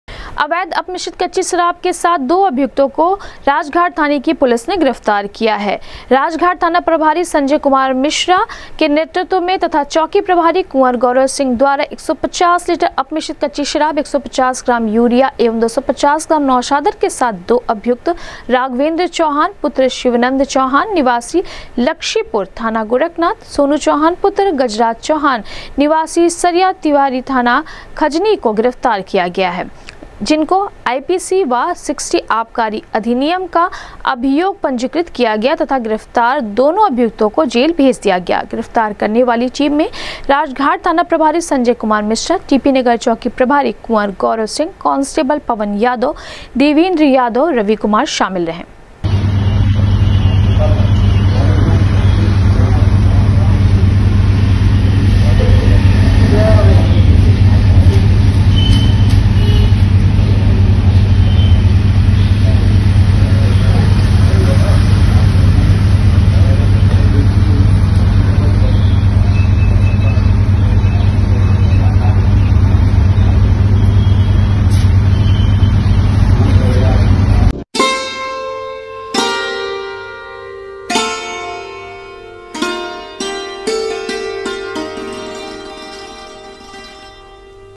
अवैध अपमिश्रित कच्ची शराब के साथ दो अभियुक्त राघवेंद्र चौहान व सोनू चौहान को राजघाट थाने की पुलिस
अवैध अपमिश्रित कच्ची शराब के साथ दो अभियुक्त राघवेंद्र चौहान व सोनू चौहान को राजघाट थाने की पुलिस ने किया गिरफ्तार, 150 लीटर अपमिश्रित कच्ची शराब, 250 ग्राम नौसादर व 150 ग्राम यूरिया किया बरामद,भेजा जेल